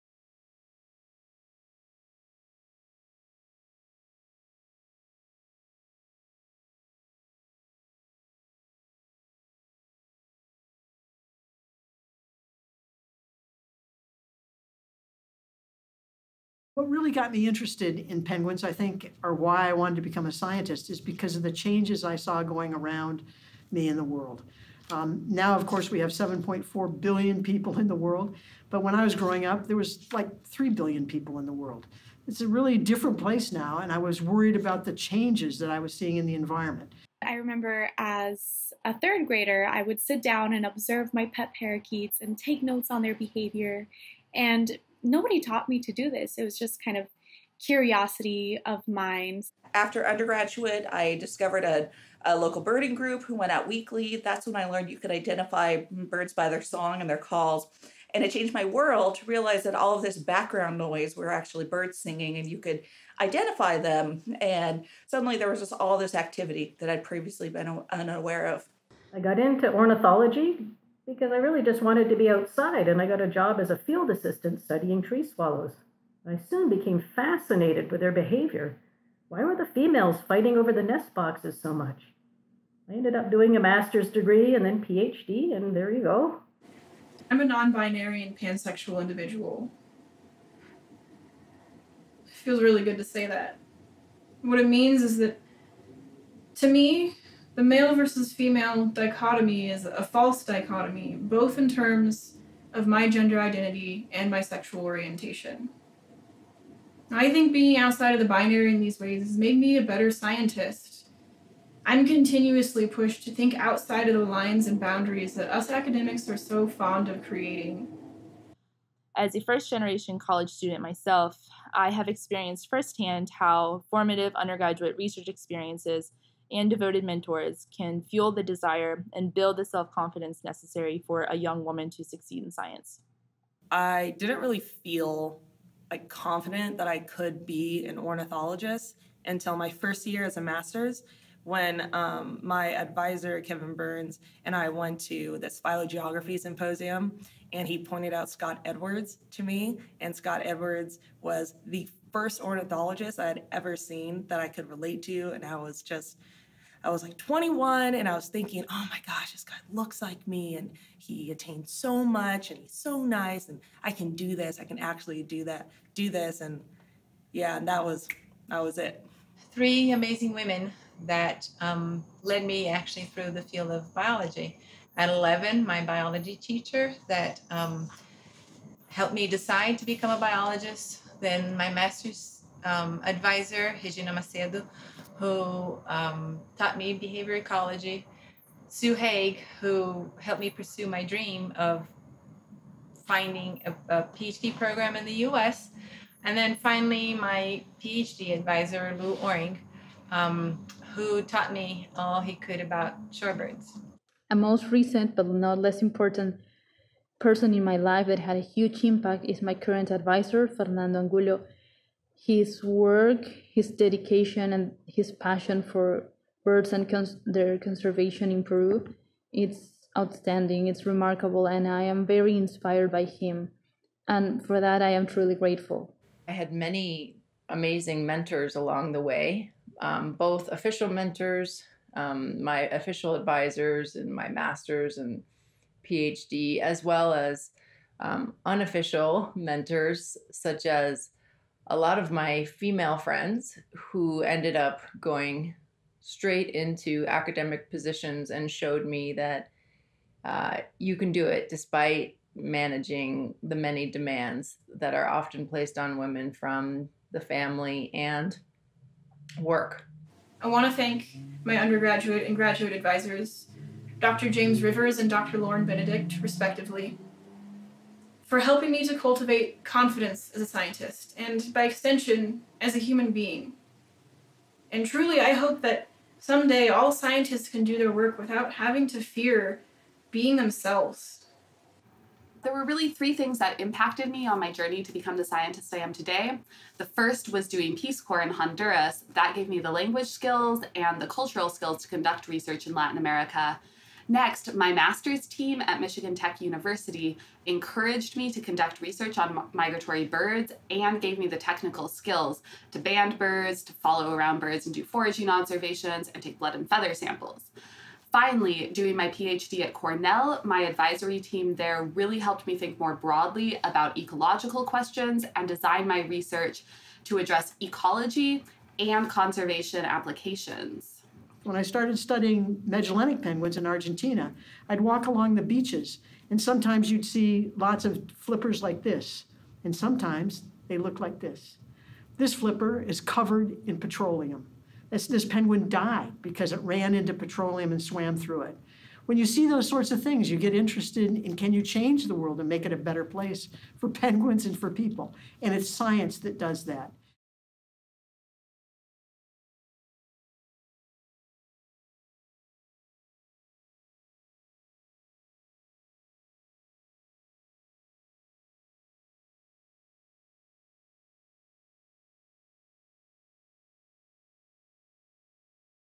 Who or what influenced you to become a scientist?
For many women and trans and non-binary people, gender has historically been a barrier to full participation in ornithology. For our 2019 meeting in Anchorage, Alaska, the American Ornithological Society worked with the Conservation Media Program at the Cornell Lab of Ornithology to produce three short videos of contemporary ornithologists telling the stories of themselves as scientists. Our objectives were to highlight diverse profiles of scientists, to show viewers that diversity is a welcome and indispensable part of a strong science community, and to contribute to the Diversity and Inclusion Committee's work to reduce barriers to conference attendance.